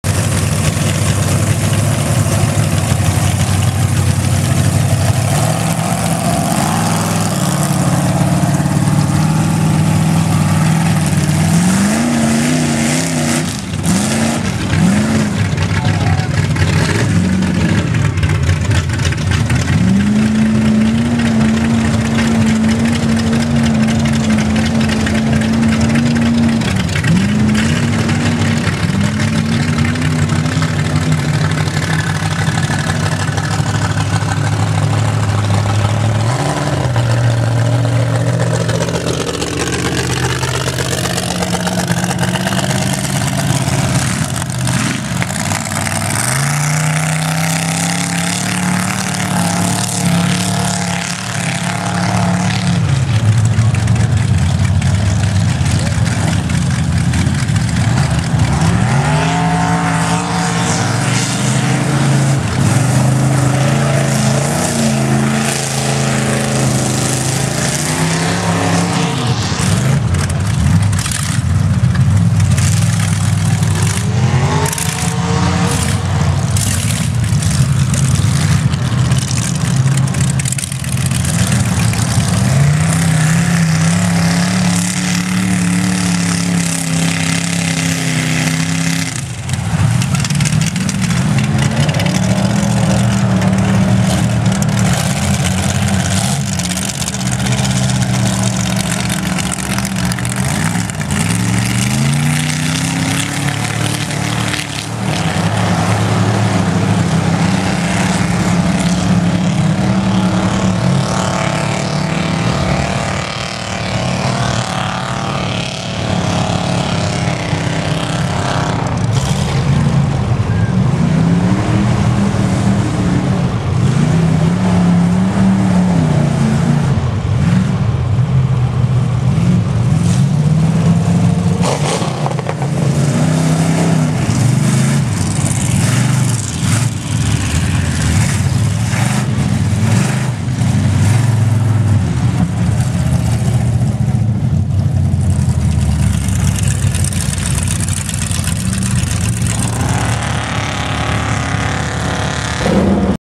MUD BOG TOWN RD PARMA MI 7/26/14
JEFF LOOPER AN JOE NELSO PLAYIN